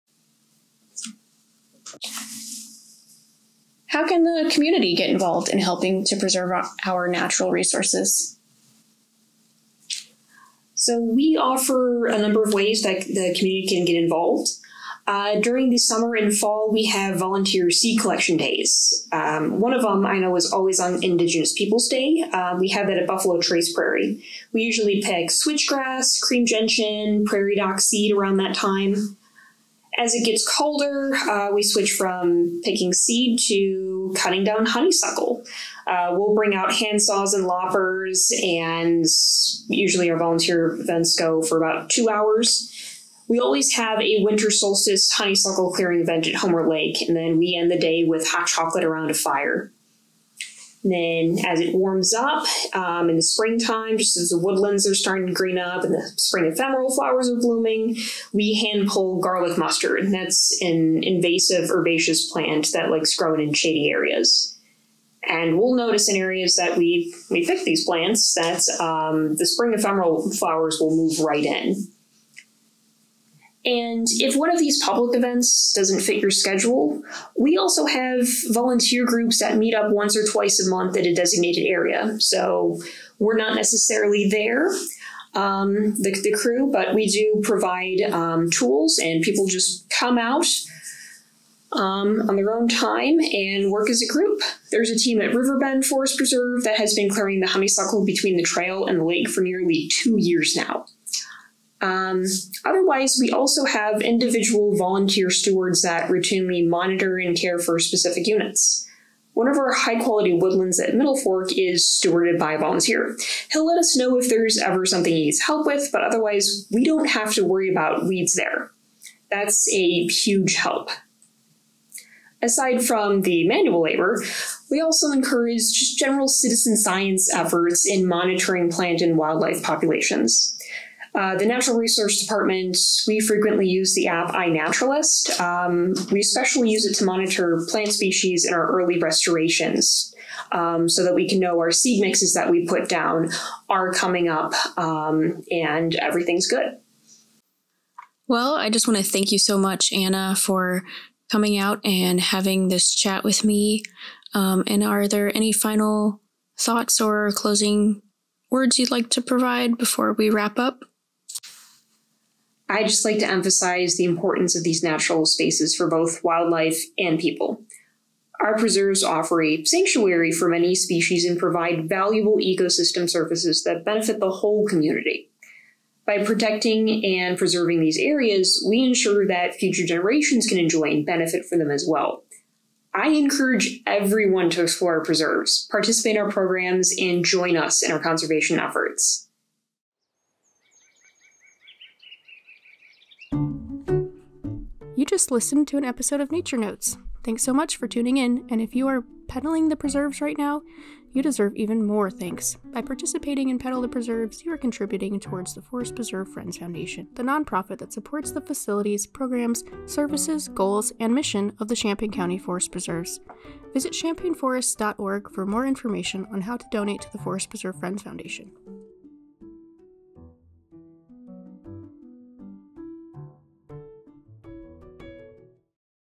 Nature Notes Episode_2-9
This is Nature Notes- a short podcast broken into bite-sized audio clips for the Pedal the Preserves event. In each brief episode you hear insights about the Champaign County Forest Preserves. Thanks for listening and Pedaling the Preserves.